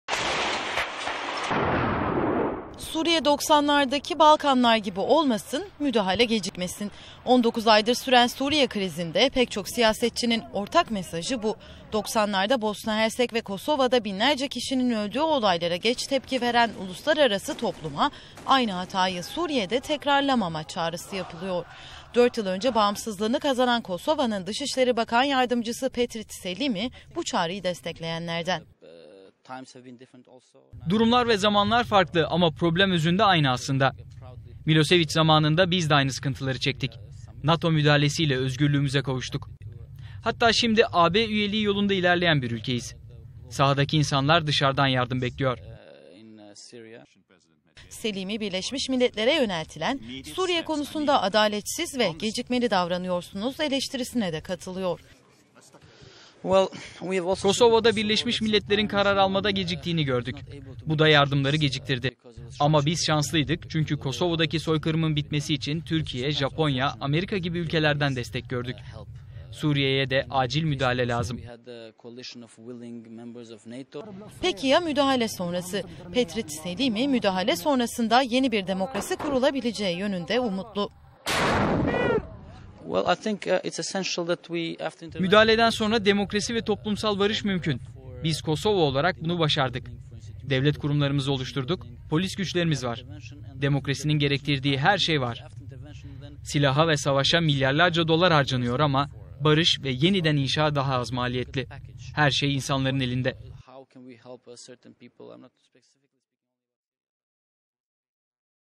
"Suriye, Balkanlar gibi olmasın"
Suriye krizi 90'larda Balkanlarda yaşanan olaylarla karşılaştırılıyor. Balkanların en genç demokrasisi Kosova'nın Dışişleri Bakanı Yardımcısı Petrit Selimi önemli mesajlar verdi. Elif Özgen ve Caner Emre Kınacı'nın haberi...